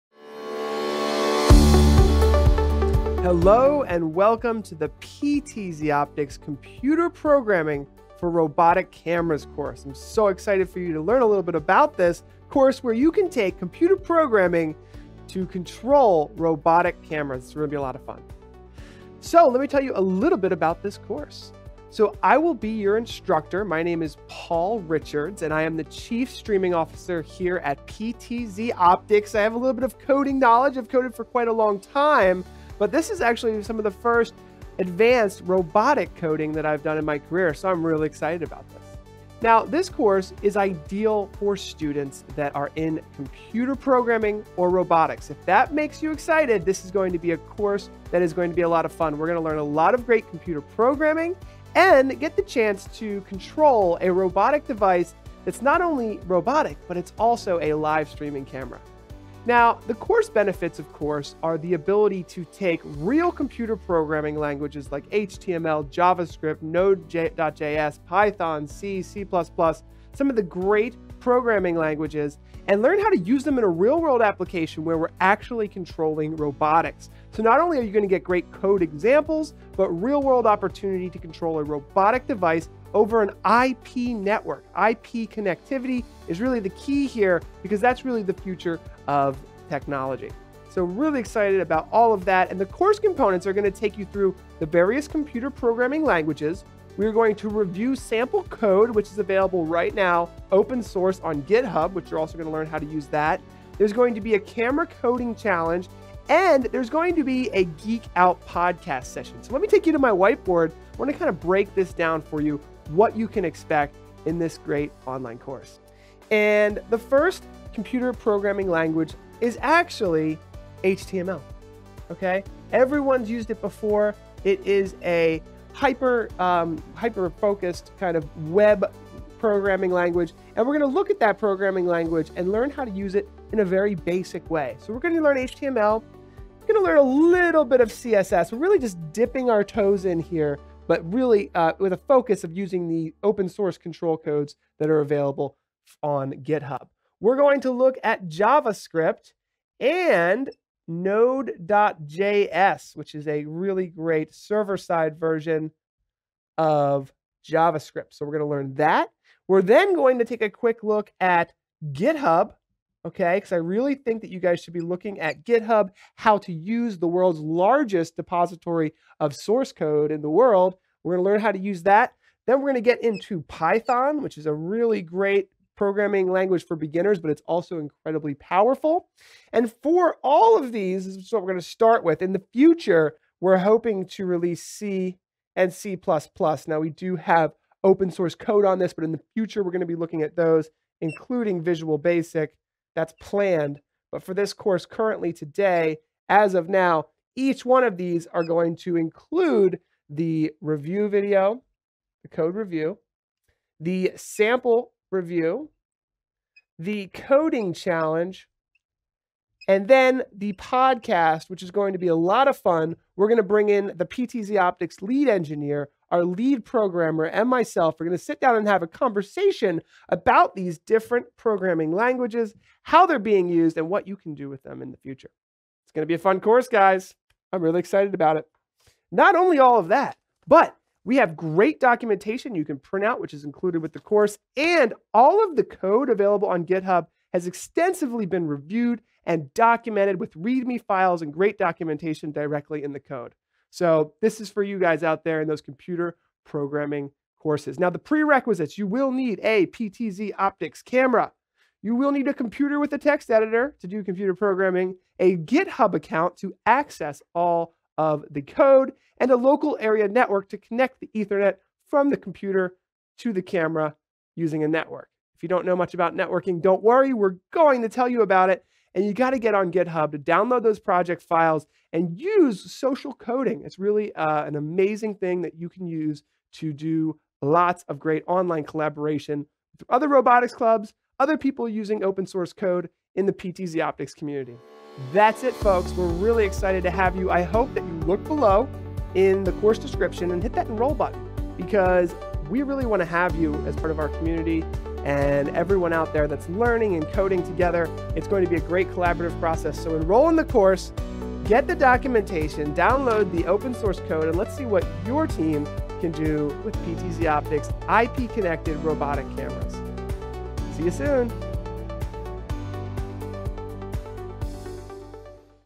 Computer Programming for Robotics Course Promo
Welcome to the PTZOptics Computer Programming for Robotic Cameras guide. This guide and included course will review computer programming languages used to control the robotic movements of PTZOptics cameras. Students will learn multiple methods of remotely controlling robotic actions with a sample IP connected robotic camera and HTTP protocols. Using IP protocols and connectivity methods, students will use standard programming languages such as HTML, JavaScript, Node.js, Python, C, and C++, to take control of precision robotics. This course is supported by a managed GitHub repository of sample codes which include detailed readme documentation. Students can refer to GitHub.com/ptzoptics anytime to access the downloadable open source codes and fork their own versions of new projects. Please reference the PTZOptics Computer Programming Grant Program for more information about obtaining a PTZOptics camera with special educational or research purposes.

 Below is a list of programming languages discussed in this course:

HTML
JavaScript
Node.js
Python
C (Coming Soon)
C++ (Coming Soon)
Visual Basic (Coming Soon)

Each unit of this course will review a different programming language. Each programming language unit will include a review video discussing the pros and cons of each programming method as they relate to robotic camera control. Each segment will consist of a C4 (Camera Control Coding Challenge) segment which will be used for students to test their knowledge on each subject. Finally, in-classroom discussion topics are provided for the full “flipped classroom” blended learning experience. Students are encouraged to watch the “Geek Out Podcast” session with concluding thoughts from the course authors, engineers, and computer programmers, available in the include online Udemy learning course.

Each course segment will include:

Programming Language Overview
Example Code Review
A Camera Control Coding Challenge
Podcast Geek Out Session

 By the end of the PTZOptics Computer Programming for Robotic Camera Course, students will have a better understanding of common computer programming languages used for remotely controlling robotic devices over an IP network. Using a fun live streaming IP-connected camera from PTZOptics, students will have the ability to write custom computer programs which can include live video and robust control over robotic actions. Each example lesson will increase in complexity, as students progress from basic HTML into the more advanced languages.

Course Prerequisites

This course will require the following:

A PTZOptics Camera
A computer with a text editor
A GitHub account
A local area network

The following videos and course segments will start with the basics of camera controls over an IP network. The course will then move through each programming language, explaining the unique properties of each. Follow the programming language overview sessions, the course will review the important of GitHub as a central “social coding” destination for your projects. From here the course will dig into the well documented code provided by PTZOptics available via GitHub. Let’s get started!